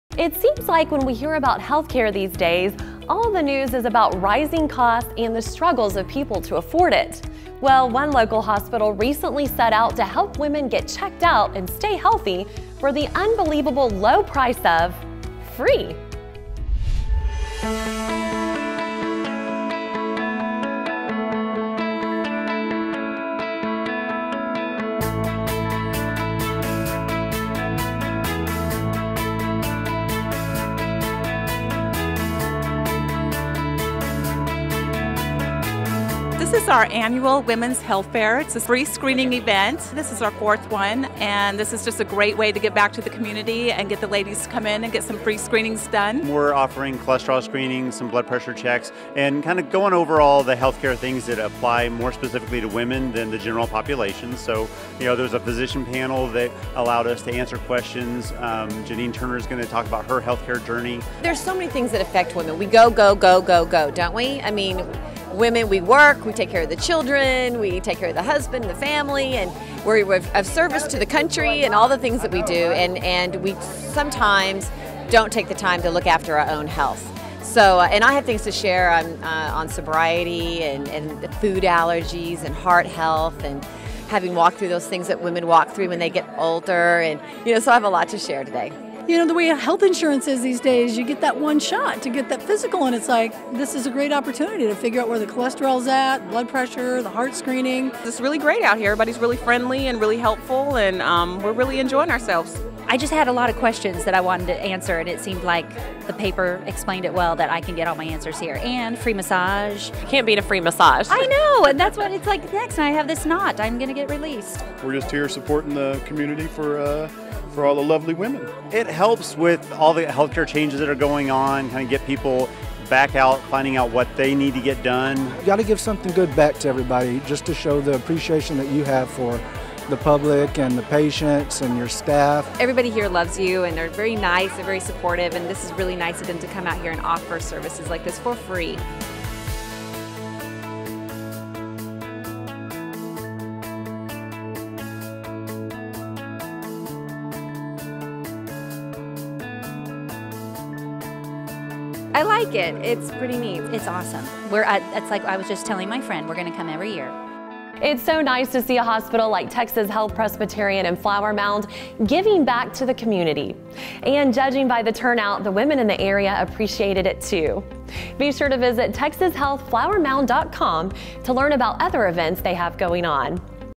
Today With Kandace - Texas Health Presbyterian Women's Health Screenings (Flower Mound, TX)
We visit Texas Health Presbyterian in Flower Mound, Texas for their annual Women's Health Fair where they offer free health screenings.